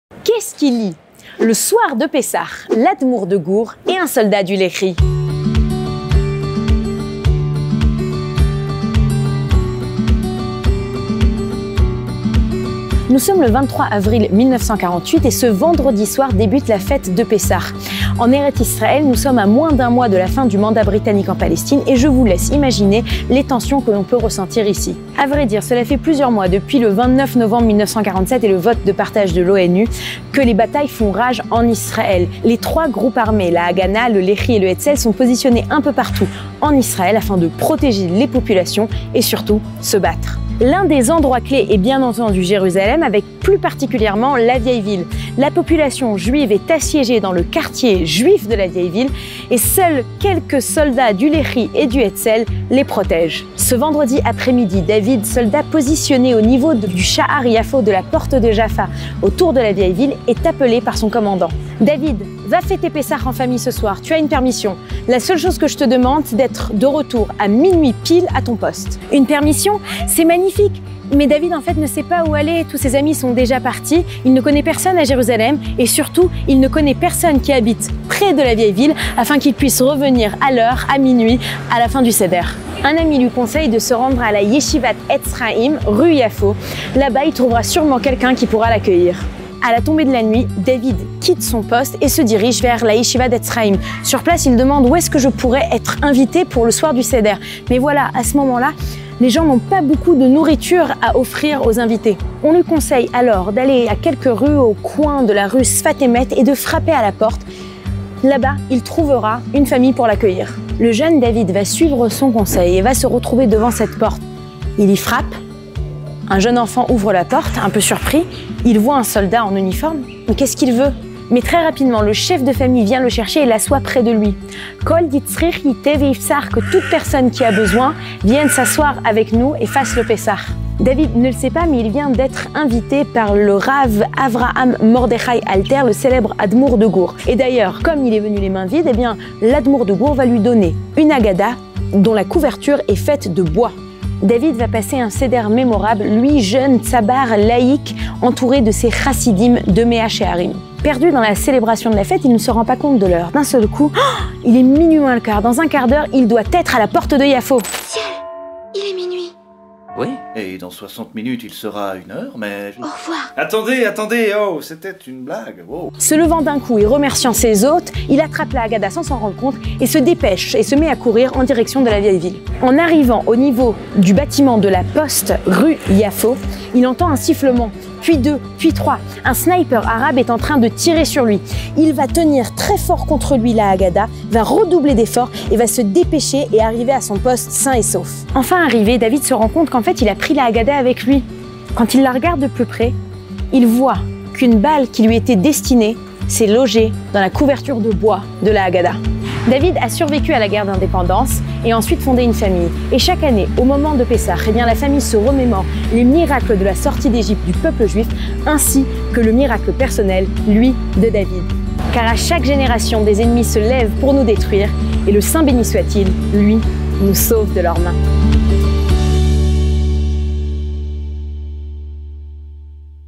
Sauvé par sa Haggadah ! Un jour notre histoire
Le Pessah 1948 a été très différent pour tous en Erets Israël et en particulier pour un soldat du Le’hi au destin bien singulier. Suivez Éliana Gurfinkiel sur les traces de sa très belle histoire.
Téléchargez des maintenant l’application studio Qualita

Rejoignez la communauté d’entraide francophone en Israël